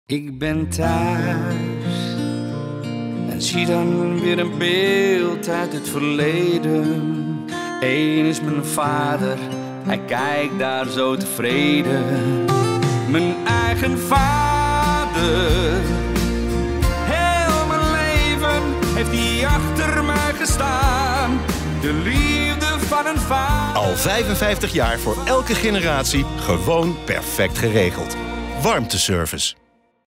Al 55 jaar voor elke generatie gewoon perfect geregeld - Warmteservice
Zo vader, zo zoon!

Ga mee terug in de tijd en ontdek hoe het er 55 jaar geleden aan toe ging. Laat je inspireren door verhalen van branchegenoten en de veranderingen door de jaren heen. Een ode aan het vakmanschap!